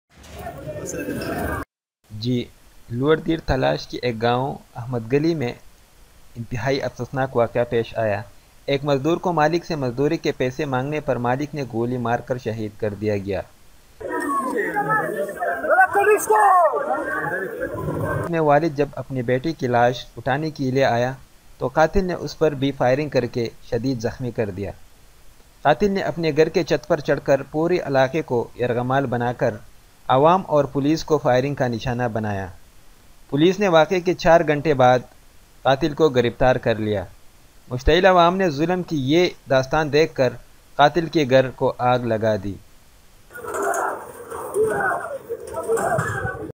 Dir lower/ Talash افسوسناک واقعہ
لوئر دیر تالاش/ افسوسناک واقعہ تالاش کے گاؤں احمد گلئ میں انتہائی افسوسناک واقعہ پیش آیا، ایک مزدور کو مالک سے مزدوری کے پیسے مانگنے پر مالک نے گولی مار کر شہید کر دیا گیا، بعد میں والد جب اپنے بیٹے کی لاش اٹھانے کےلئے آیا تو قاتل نے اس پر بھی فائرنگ کر کے شدید زخمی کردیا۔ قاتل نے اپنے گھر کے چھت پر چڑھ کر پورے علاقے کو یرغمال بنا کر عوام اور پولیس کو فائرنگ کا نشانہ بنایا۔ پولیس نے واقعے کے 4 گھنٹے بعد قاتل کو گرفتار کر لیا۔ مشتعل عوام نے ظلم کی یہ داستان دیکھ کر  قاتل کے گھر کو آگ لگا دی